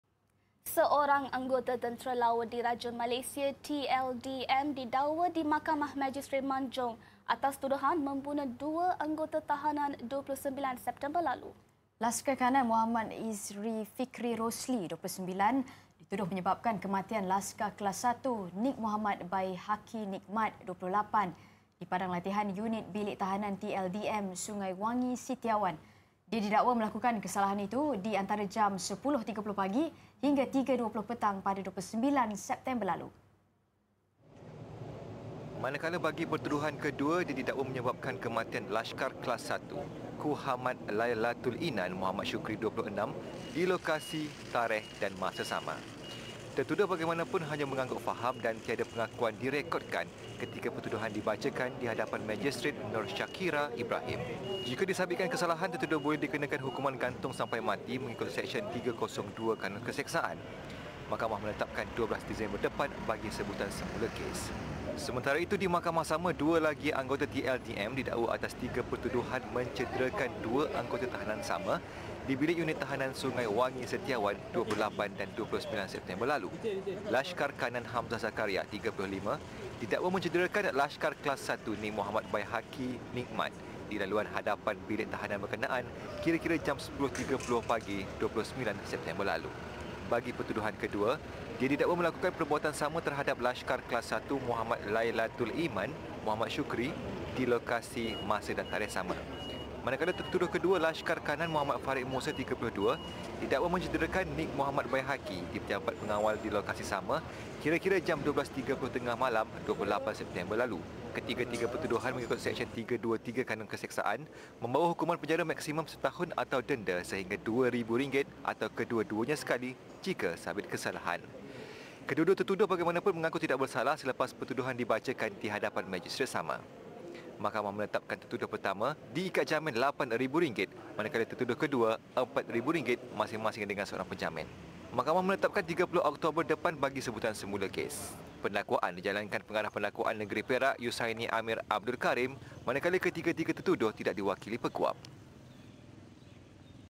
Seorang Anggota TLDM Didakwa Bunuh 2 Anggota Tahanan
Seorang anggota Tentera Laur Diraja Malaysia (TLDM) didakwa di Mahkamah Majistret Manjung atas tuduhan membunuh dua anggota tahanan, 29 September lalu.

Laskar Kanan Mohd Izhrifikri Roslee, 29 tahun, dituduh menyebabkan kematian Laskar Kelas 1 Nik Muhammad Baihaqy Nik Mat ,28 tahun, di Padang Latihan Unit Bilik Tahanan TLDM Sungai Wangi Sitiawan.